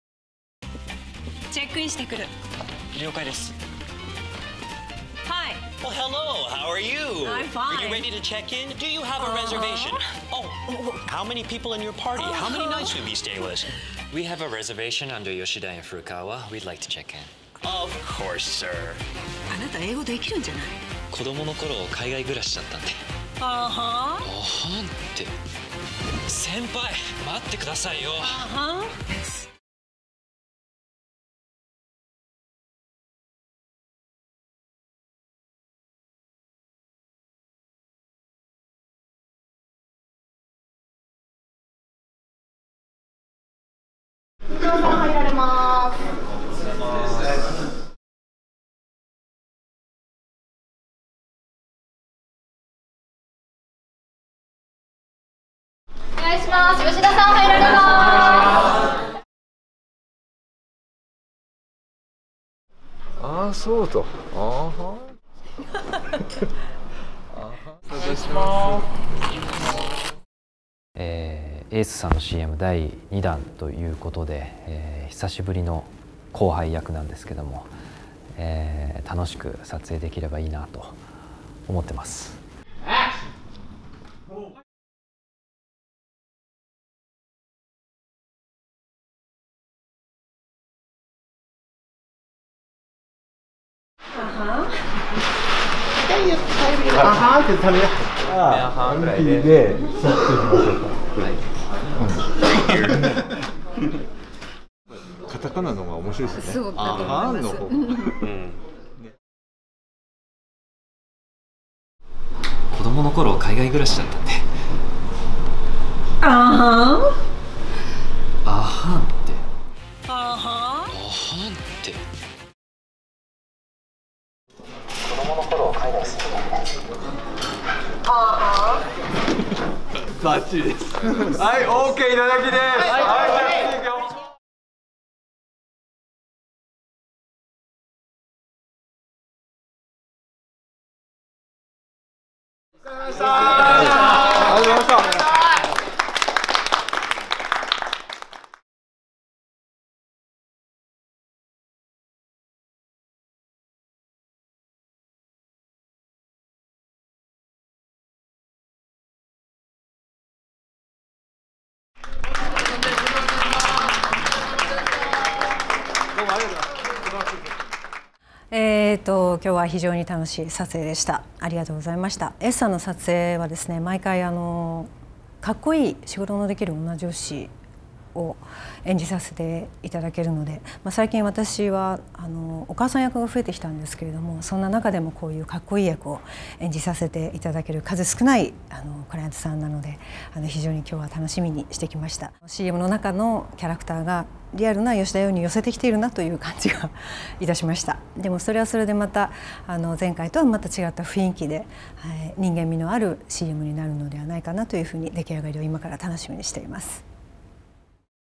吉田羊、古川雄輝の流暢な英語に驚き バッグ＆ラゲージブランド『ace.』新CM「チェックイン」篇＆メイキング映像
女優の吉田羊と俳優の古川雄輝が28日より公開される、バッグ＆ラゲージブランド『ace.』の新CMに出演。海外出張中、ホテルにチェックインしようとする先輩・吉田が英語での会話に苦戦する中、見かねた後輩役の古川が流暢な英語でスムーズにチェックインを行う。

小松菜奈＆吉田羊が6変化「普段着ることないですね」キャリアウーマン姿のレアショット公開